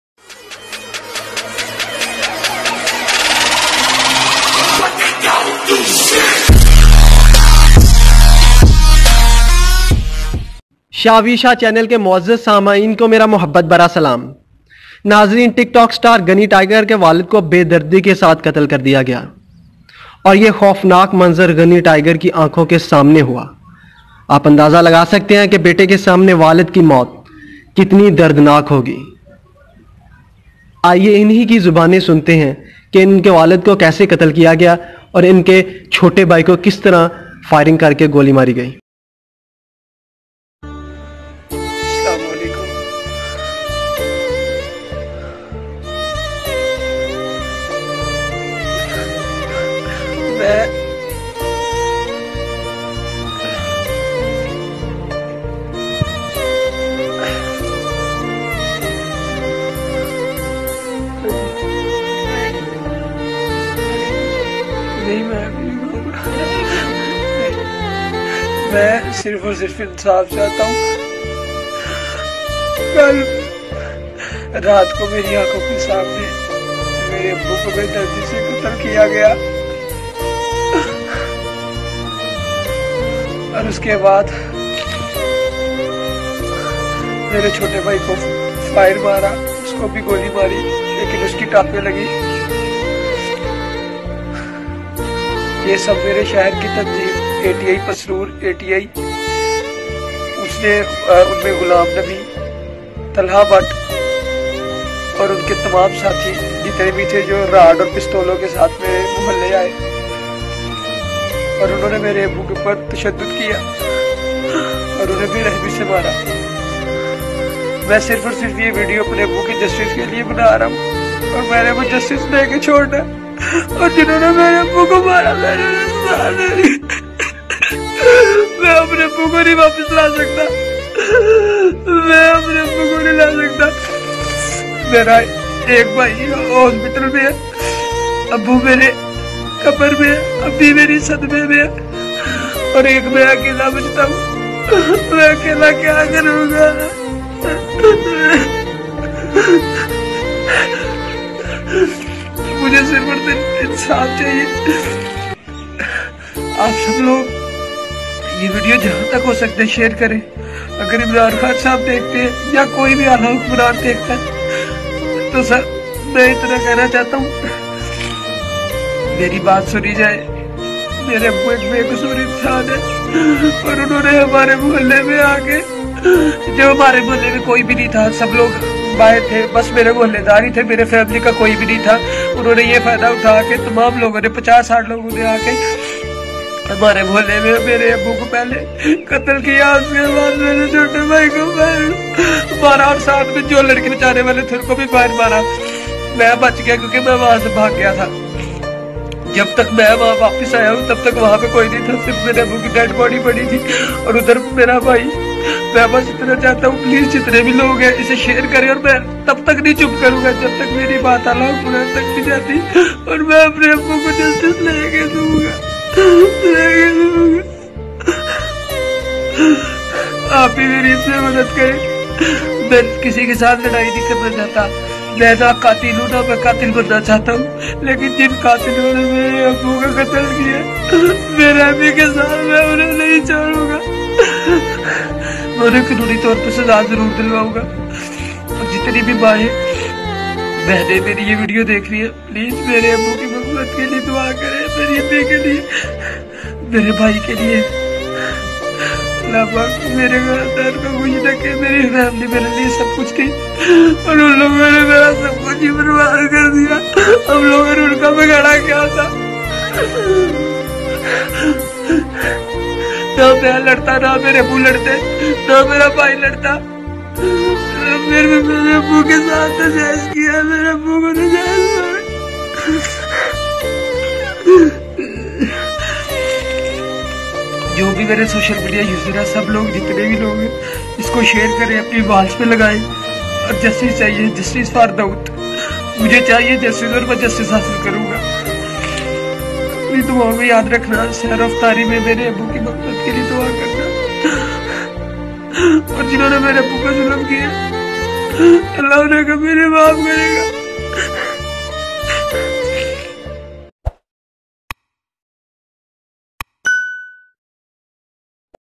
Ghani Tiger (Hamza) Father Story | Culprits Exposed | Justice For Dawood
Ghani Tiger is famous tik tok star and rapstar of Pakistan.